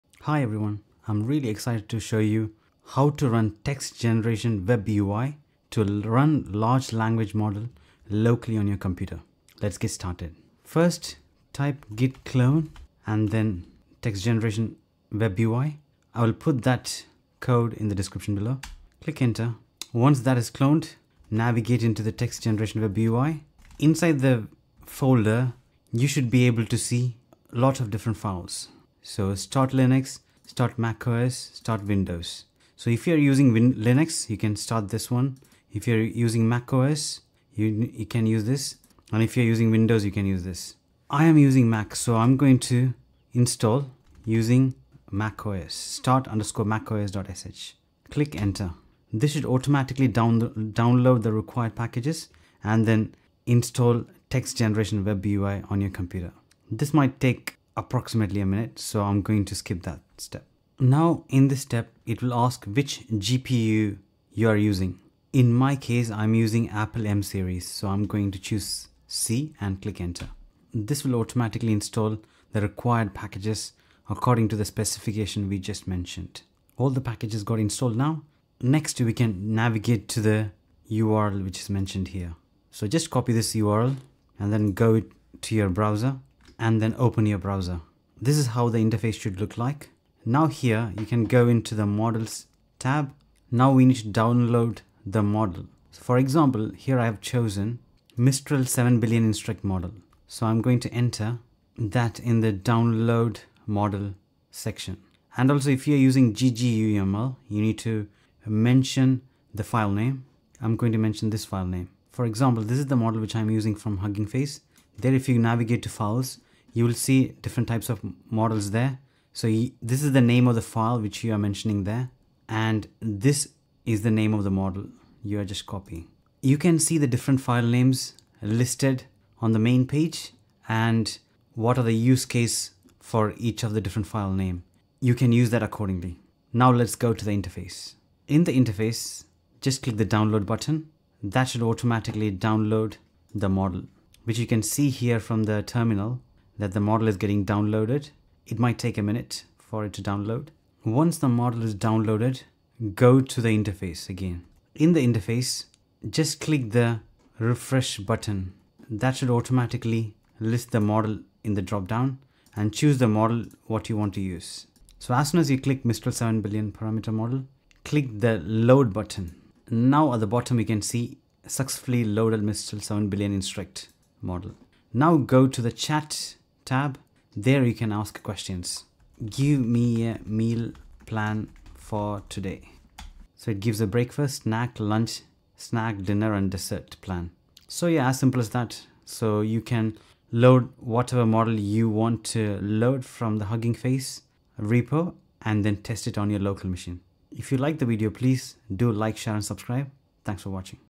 Text Generation Web UI: MIND-BLOWING Way to Run LLM Locally! 🤯
The Text Generation Web UI is designed to be a versatile interface for text generation models. It supports multiple backends, offers various interface modes, and includes advanced features like LoRA and Markdown output with LaTeX rendering. Installation involves cloning the repository and running an OS-specific start script.

### Commands for Setup
1. Clone repository: `git clone
2. Navigate to directory: `cd text-generation-webui`
3. Run OS-specific script:
   - Linux: `./start_linux.sh`
   - Windows: `start_windows.bat`
   - macOS: `./start_macos.sh`
   - WSL: `./start_wsl.bat`
4. Select GPU vendor when prompted.

```bash
# Combine all commands
cd text-generation-webui
# Replace [OS_SCRIPT] with your OS-specific script
./[OS_SCRIPT]
To find your OS-specific script, use: `ls | grep start` and replace `[OS_SCRIPT]` with the result.

Timestamps
0:00 - Introduction
0:02 - What You'll Learn
0:05 - Running LLM Locally
0:11 - Cloning the Repository
0:20 - Navigating Inside the Folder
0:31 - Choosing Your OS
0:45 - Installation Process
0:59 - Selecting GPU
1:17 - Packages Installed
1:20 - Opening Web UI
1:30 - Downloading Models
1:45 - Specifying Model File
2:09 - Use Cases for Different Files
2:17 - Downloading the Model
2:31 - Refreshing Interface
2:42 - Choosing the Model
2:49 - Model Loaded
2:57 - Using the Chat Tab
3:09 - Loading Models from Hugging Face
3:17 - Conclusion and Call to Action